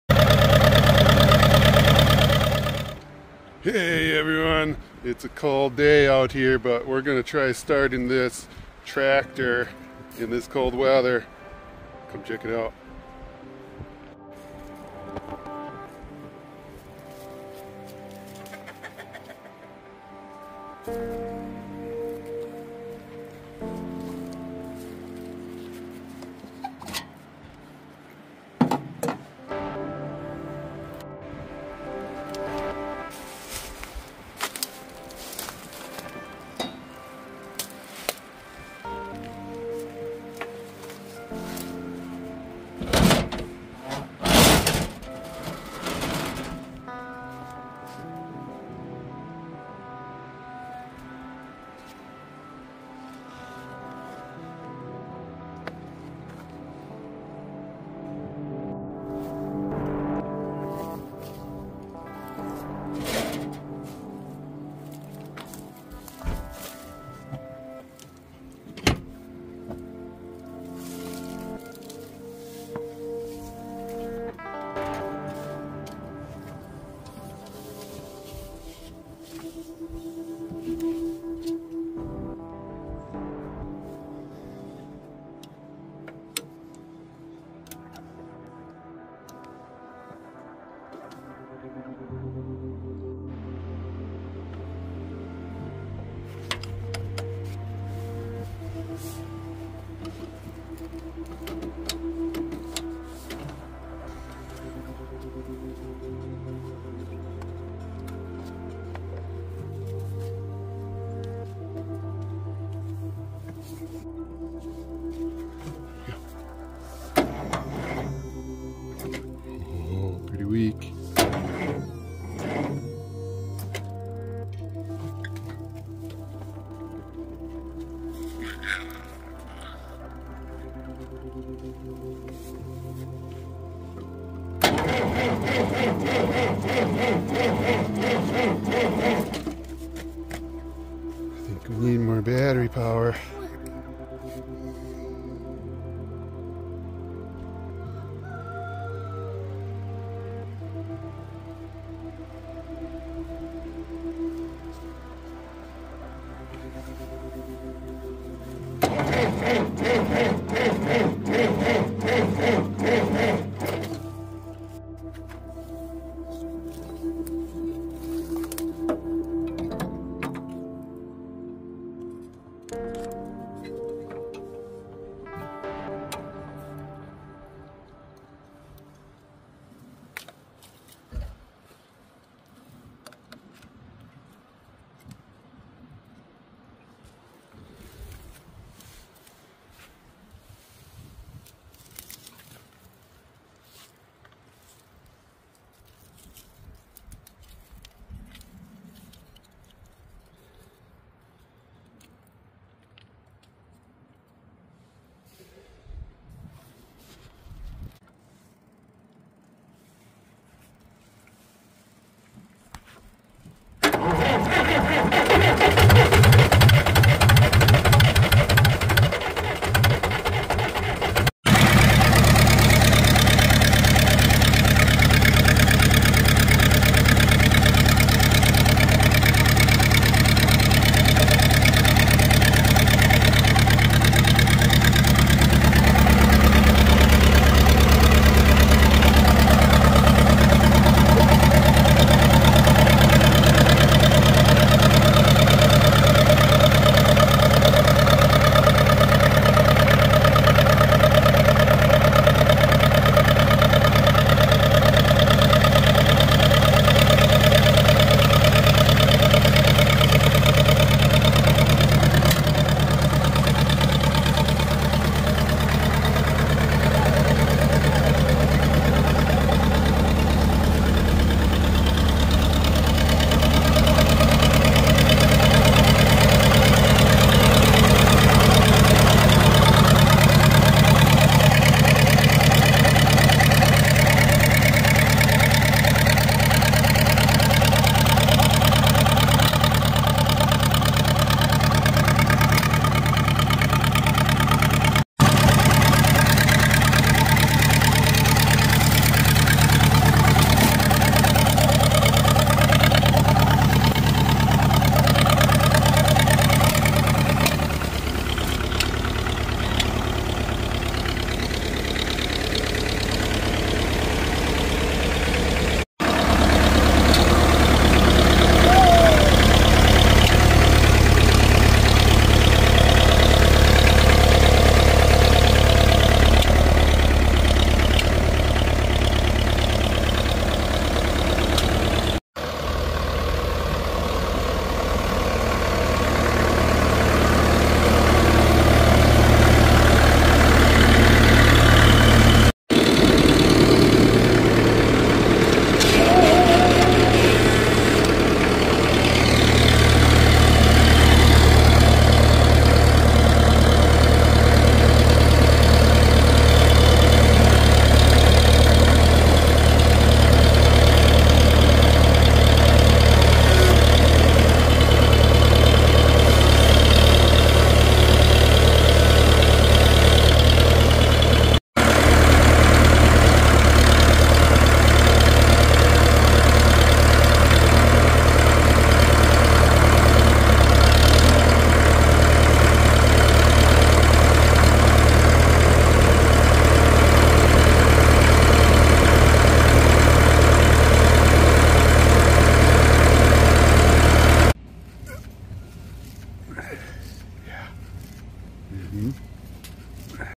Cold Start 64 year old diesel.
Starting a 1960 International B275 Diesel tractor at 27 degrees.

Not the coldest we've started it by far, but with weak batteries we needed to jump start this tractor.

This has a -12v electrical system so we connect the positive to ground.